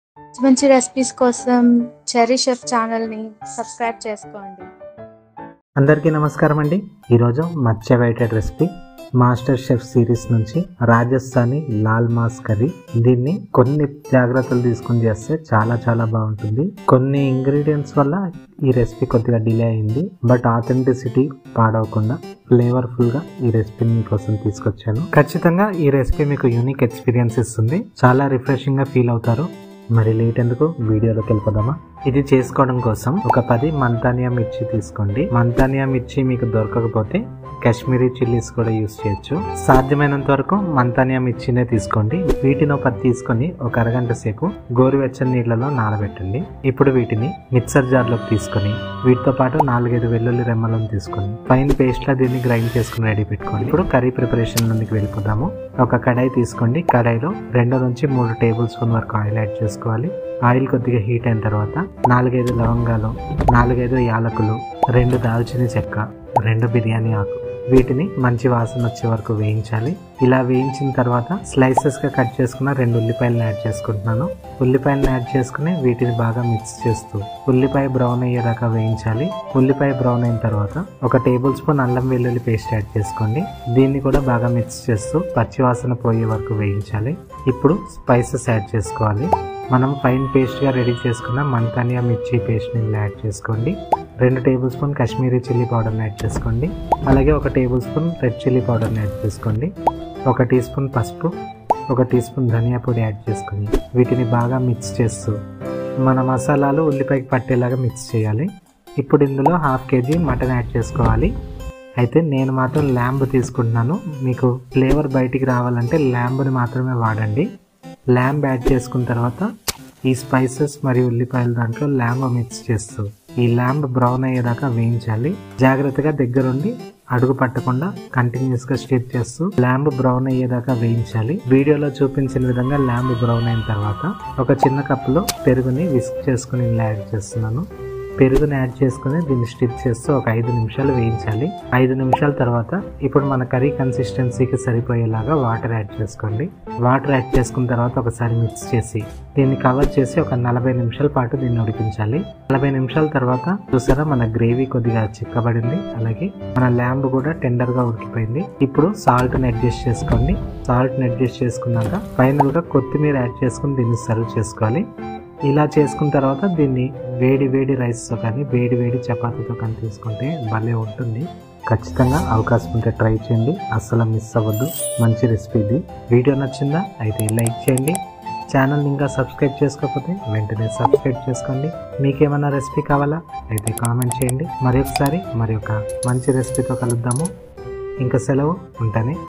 Cherry Chef | Laal Maas Recipe | Rajasthani Lamb Curry | ఈ సీక్రెట్ రిసిపీ మీకోసమే!
🔥 Laal Maas Recipe | Authentic Rajasthani Lamb Curry 🔥

Namaskaram foodies! 🙏 Today, I’m excited to bring you a special Laal Maas recipe straight from the Master Chef 2024 Telugu Version 🍽️! Thank you so much for recommending this show – it’s been an absolute eye-catcher, and I can’t wait to share more delicious recipes from it with you all! 🤩

I know this video is a little late 🕓, but trust me, it was worth the wait! I was waiting for some special ingredients to make this recipe as authentic as possible. A huge thanks to all of you for suggesting this gem of a dish – your recommendations always make my content better! 💖

If you’re all interested, I’d be delighted to bring even more exclusive recipes from Master Chef! 🍛👩‍🍳

📝Ingredients [Chilly Paste]:
1. Manthania Red Chillies x10pc (Soaked 30min in warm water)
2. Garlic Cloves x4-5pc
3. Water (as needed)

📝Ingredients [Lal Maas Curry]:
1. Oil or Ghee 2-3Tbsp
2. Cloves 4-5pc
3. Green Cardamom 4-5pc
4. Cinnamon Sticks 1-2pc
5. Black Cardamom x1-2pc (Must use if Possible)
6. Bayleaf x2
7. Sliced Onion x2pc
8. Ginger-Garlic Paste 1-2Tbsp
9. Manthania Chilly Paste
10. Kashmiri Chilly Powder 2Tbsp
11. Red Chilly Powder 1Tbsp (Adjustable)
12. Turmeric Powder 1Tsp
13. Coriander Powder 1Tsp
14. Chopped Lamb 500g
15. Whisked Curd 1cup (small)
16. Water (as needed)
17. Salt (Adjustable)
18. Cilantro 2Tbsp
19. Charcoal (Smoking-Optional)